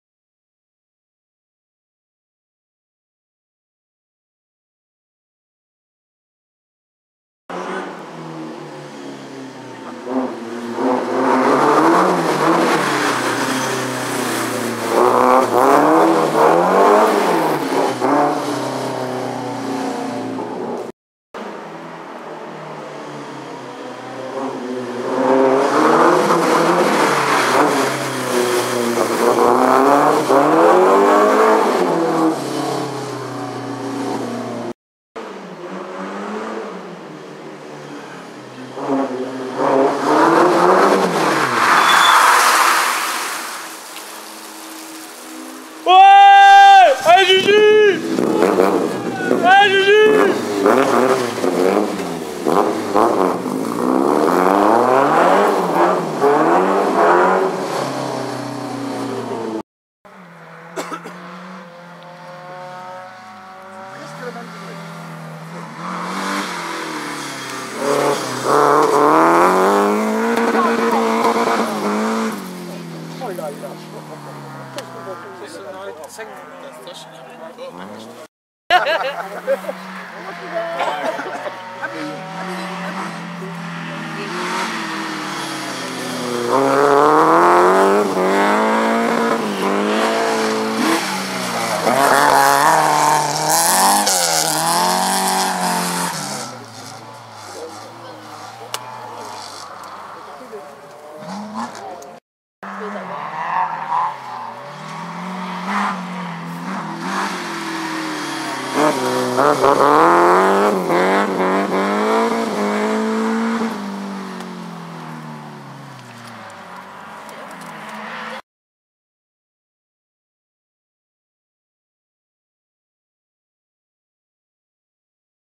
RS St Roch 2014 Basecq by DJK
voici nos amis Julien Basecq et Christophe Lemoine au bord de la manta encore du grand show !